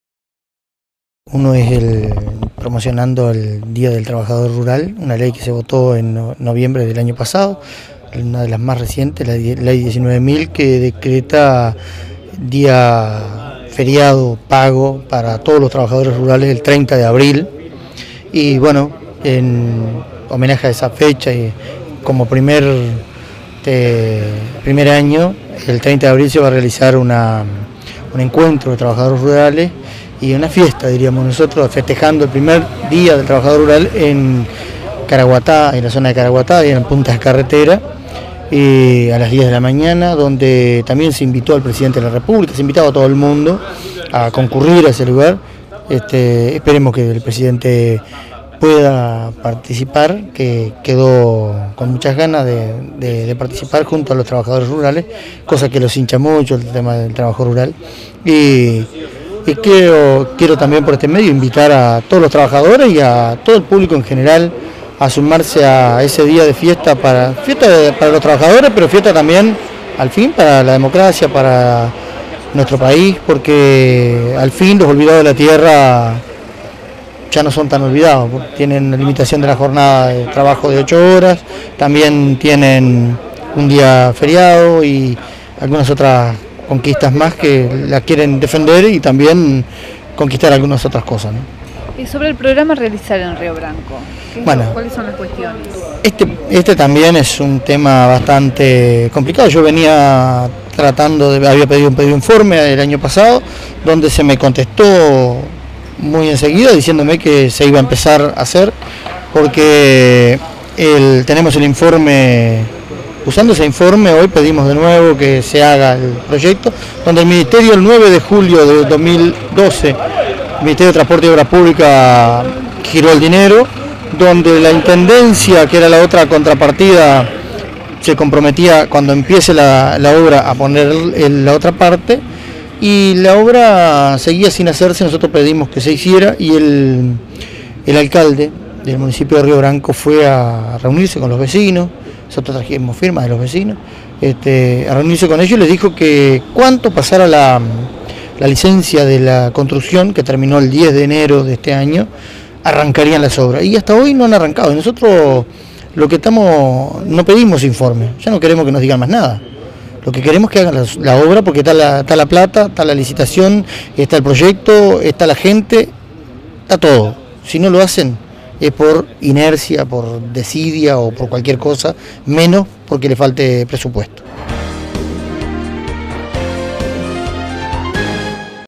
Entrevista al edil Dardo Perez - Canal 2 Cable del Lago
Entrevista sobre el día del trabajador rural y sobre el Programa Realizar 2011 en la ciudad de Rio Branco.Más informaciones